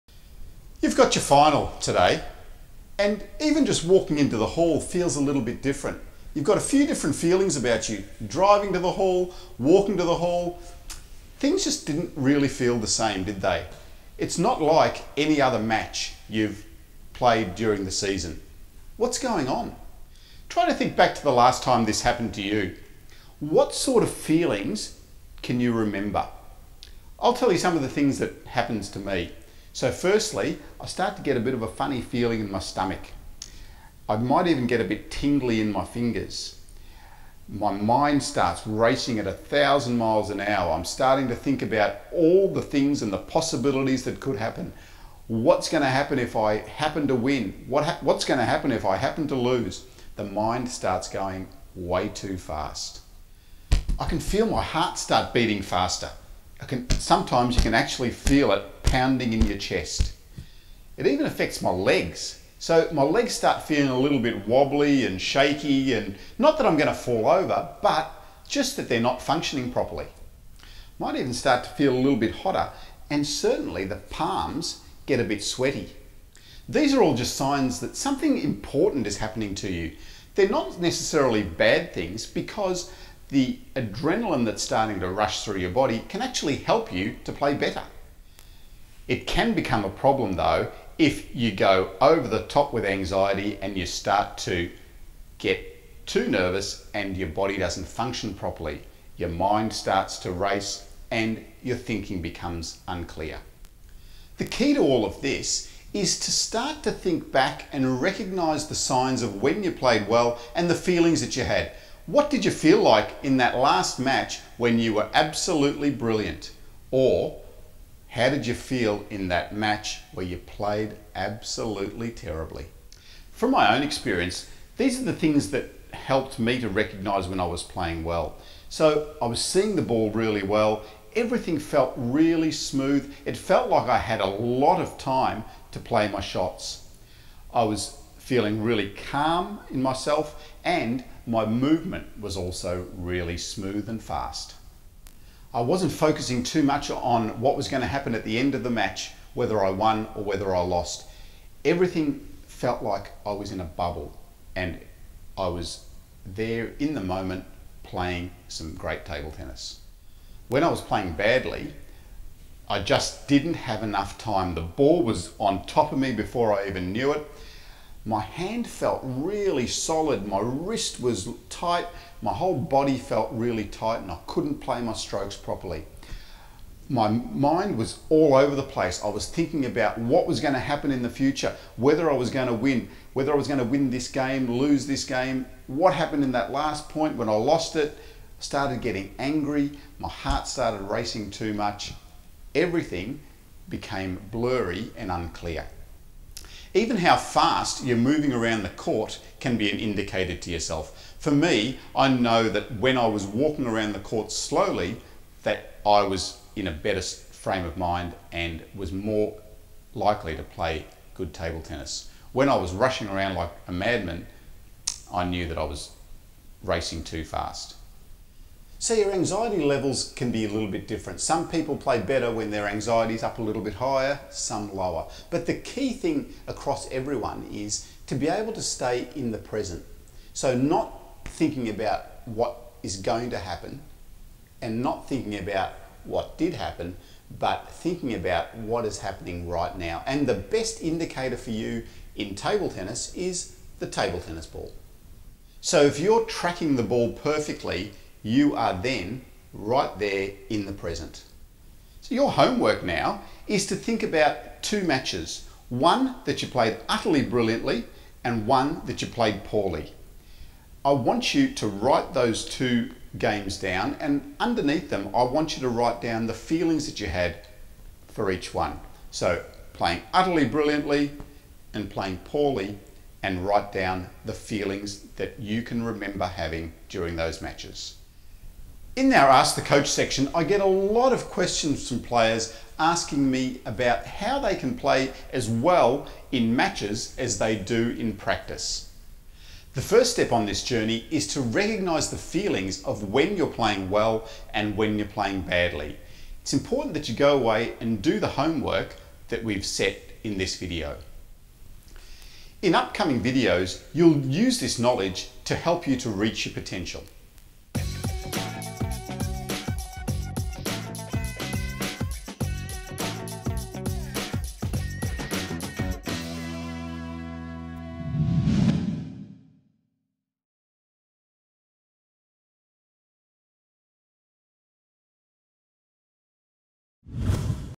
Sports Psychology - Recognise Your Feelings | Table Tennis | PingSkills
The first step into the world of sports psychology is to recognise your feelings when you are playing well and when you are playing poorly. This video talks about some of those feelings and asks you to think about different matches you have played and how you felt during those matches. In future videos on the subject we will use this information to help you reach your potential.